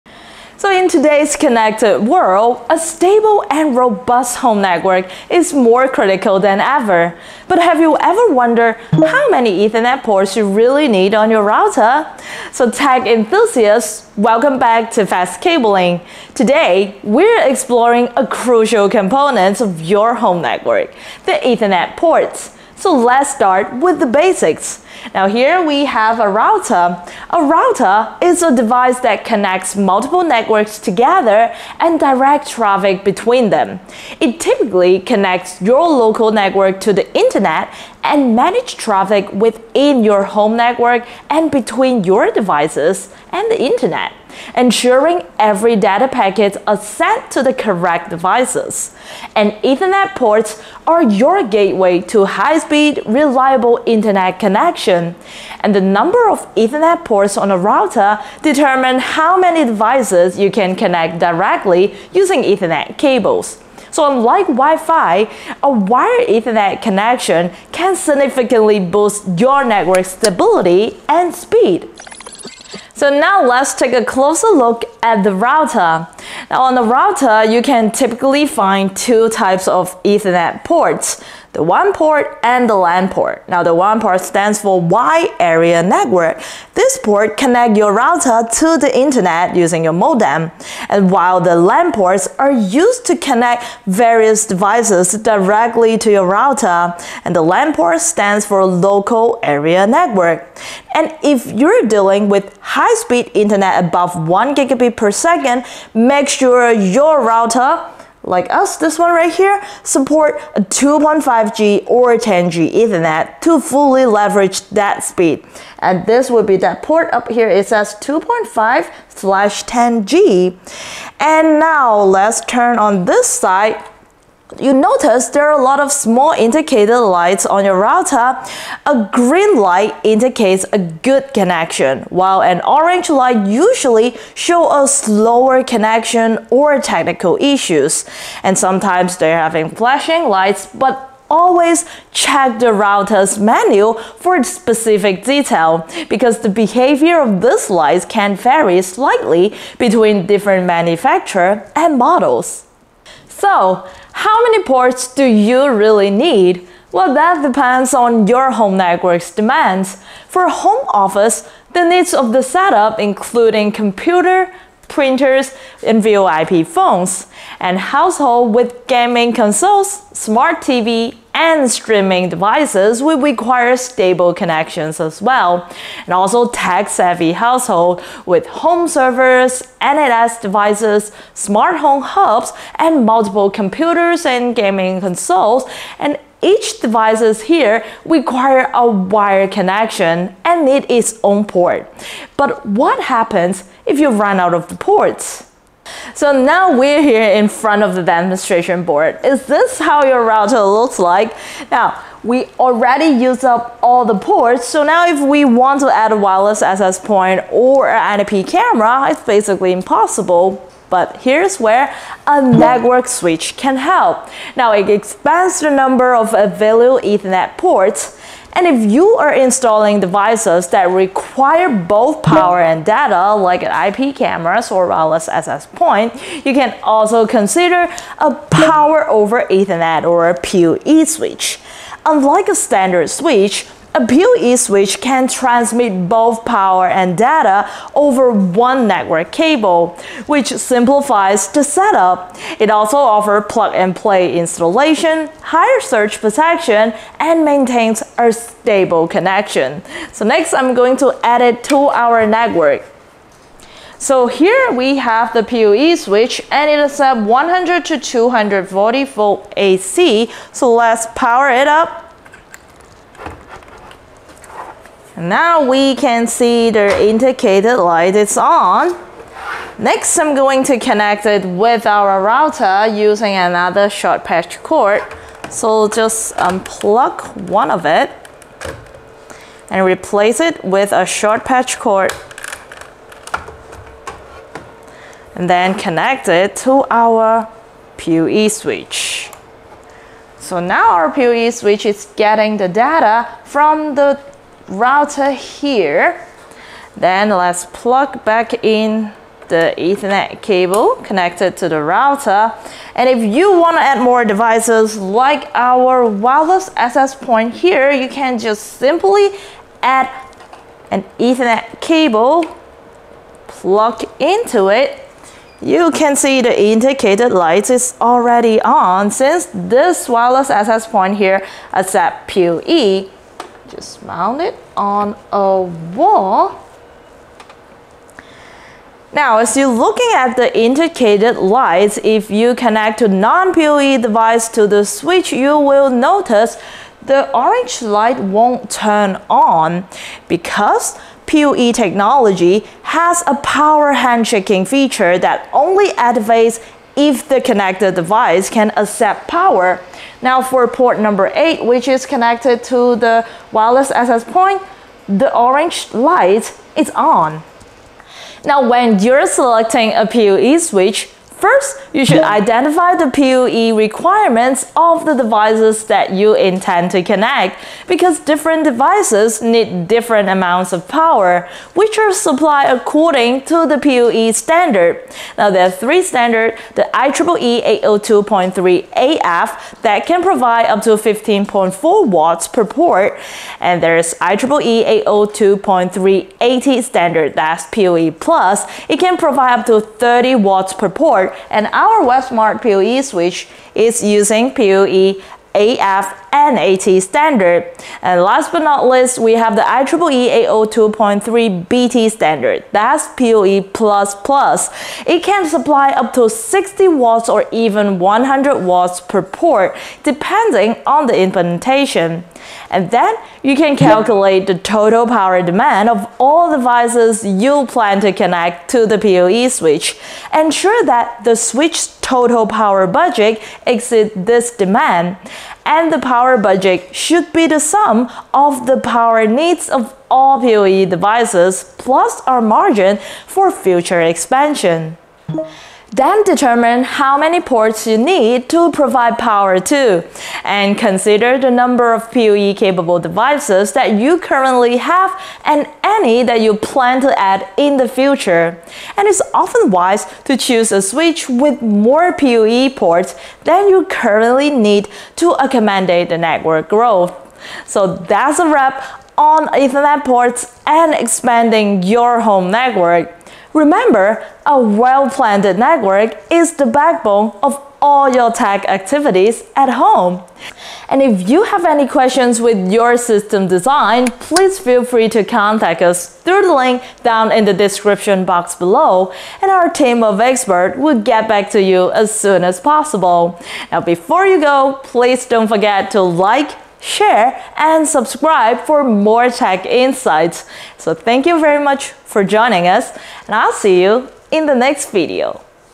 How Many Ethernet Ports Do You Really Need on Your Router?
Are you unsure about how many Ethernet ports your router should have? In this video, we delve into the world of Ethernet ports on routers and explore how many you actually need for your home network. From understanding the roles of WAN and LAN ports to assessing your network needs and expanding your ports with network switches. We introduce different types, like PoE switches, and compare their features.

0:00:00 Introduction
Understanding Ethernet Ports
Assessing Your Home Network Needs
What if Your Router has Too Few Ports?
Expanding Your Network with a Network Switch
Recommendations and tips for choosing the right switch
Need for PoE switch and power handshaking with non-PoE devices